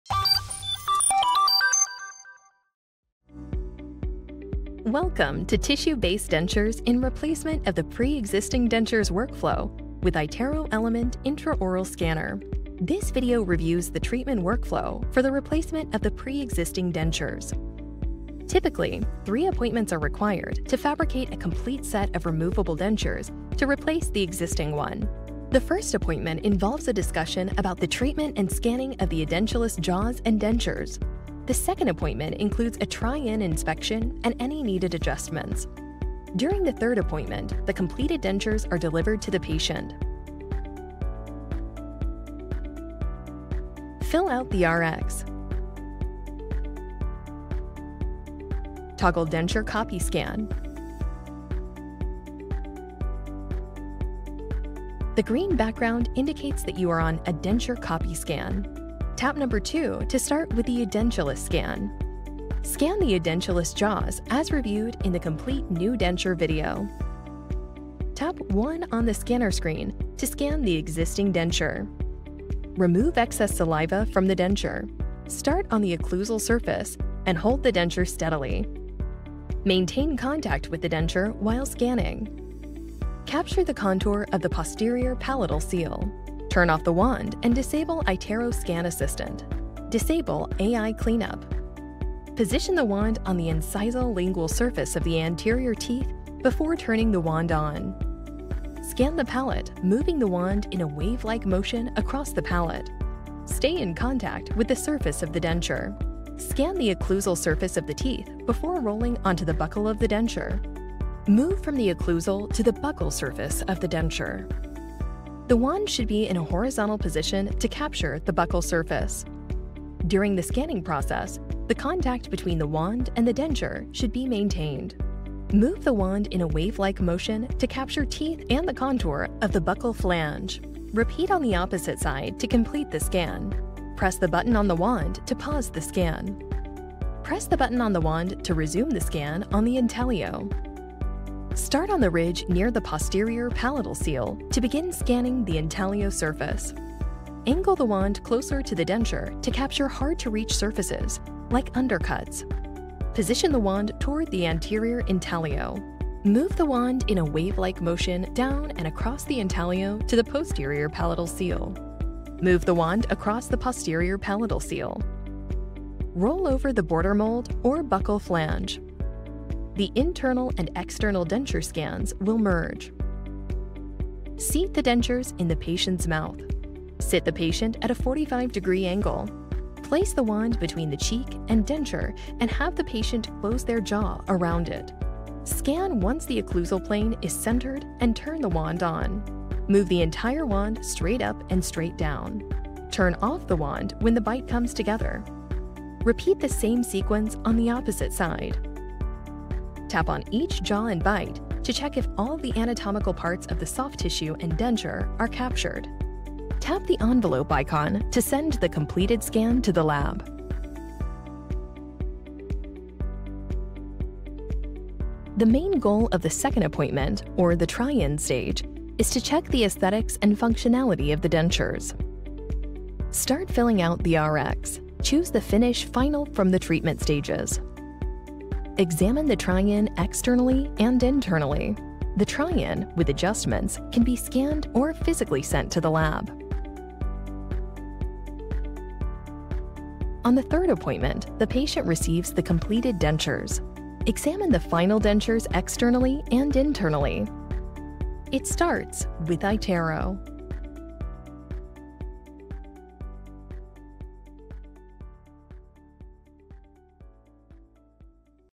How to scan dentures with the iTero Element™ Plus scanner | tissue-based workflow for replacements
Learn how to scan dentures with the iTero Element™ Plus intraoral scanner using the tissue-based workflow for replacing pre-existing dentures.
This tutorial covers:
- Step-by-step scanning of edentulous jaws and existing dentures
- Capturing internal and external denture surfaces and merging scans
- Recording the bite with the denture seated in the patient’s mouth
- Using the three-appointment workflow: initial scan, try-in, and final denture delivery

The iTero Element™ Plus scanner enables a predictable, efficient, and precise process for complete denture replacement cases.

Chapters:
0:00 Introduction
0:41 Appointment one. Treatment discussion and scanning.
3:31 Appointment two. Try-in inspection.
3:57 Appointment three. Denture delivery.